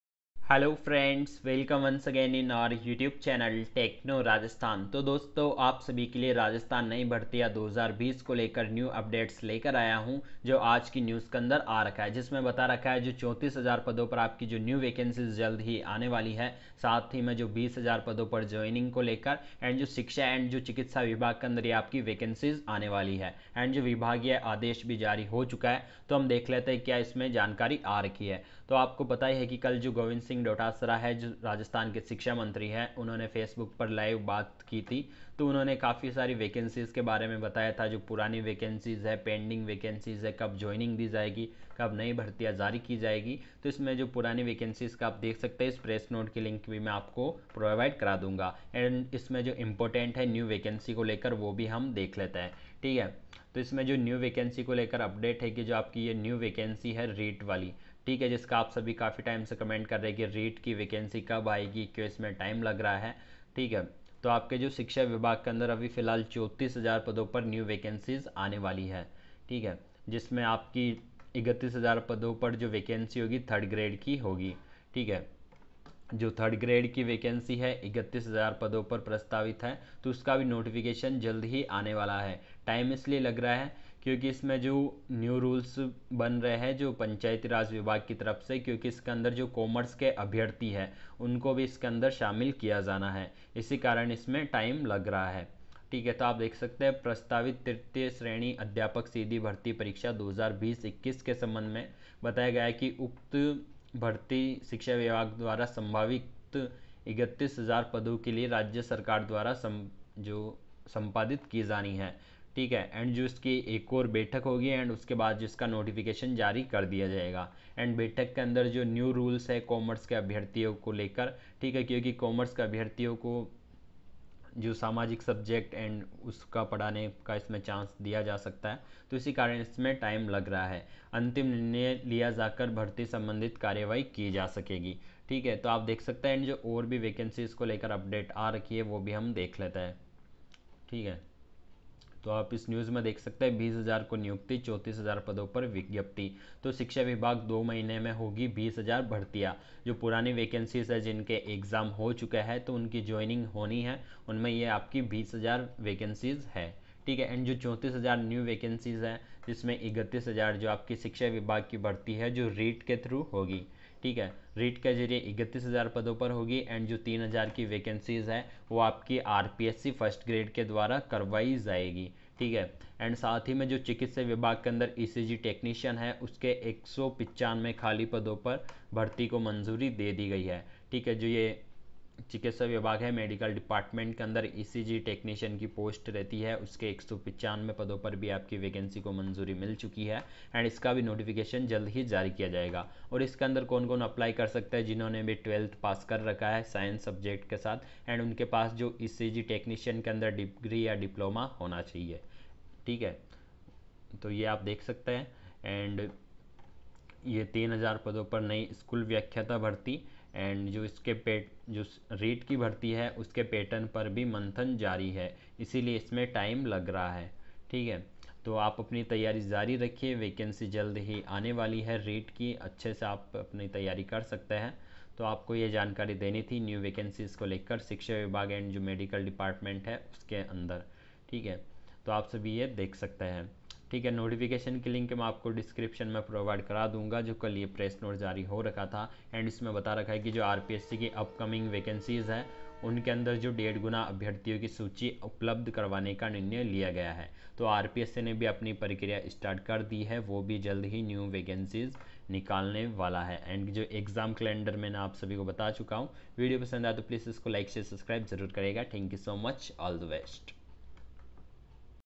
राजस्थान सरकार नई भर्तियां 2020 / 54000 पदों पर / शिक्षा विभाग चिकित्सा विभाग REET Teacher Vacancy
rajsthan Govt Recruitment Jobs, rajasthan upcoming Vacancy,
rajasthan reet teacher recruitment 2020,